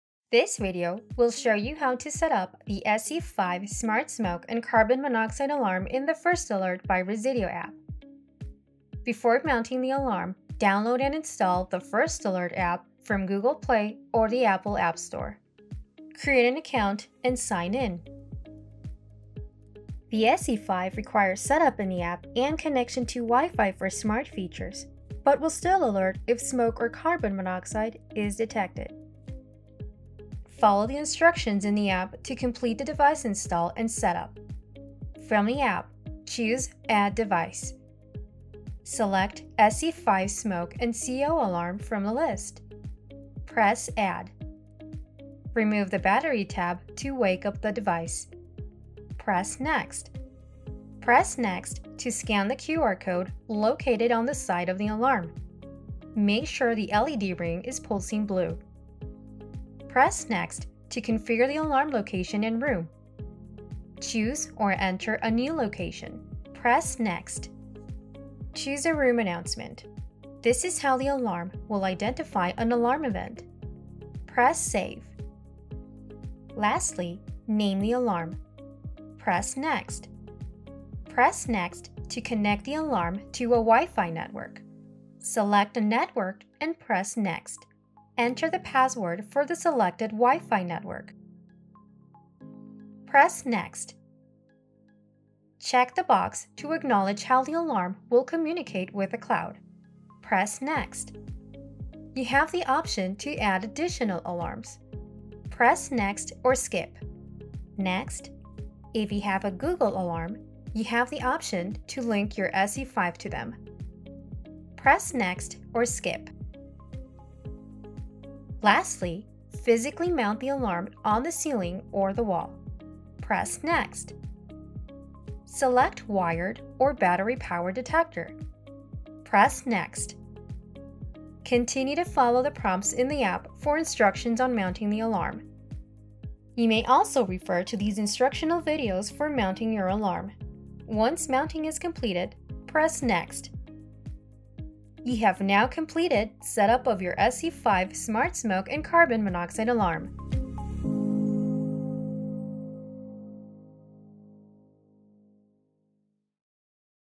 Setup and Connect the SC5 Smart Smoke and Carbon Monoxide Alarm to the First Alert App and Wi-Fi
This video will show you how to setup up the SC5 Smart Smoke and Carbon Monoxide Alarm to the First Alert app and connect the alarm to a WiFi network.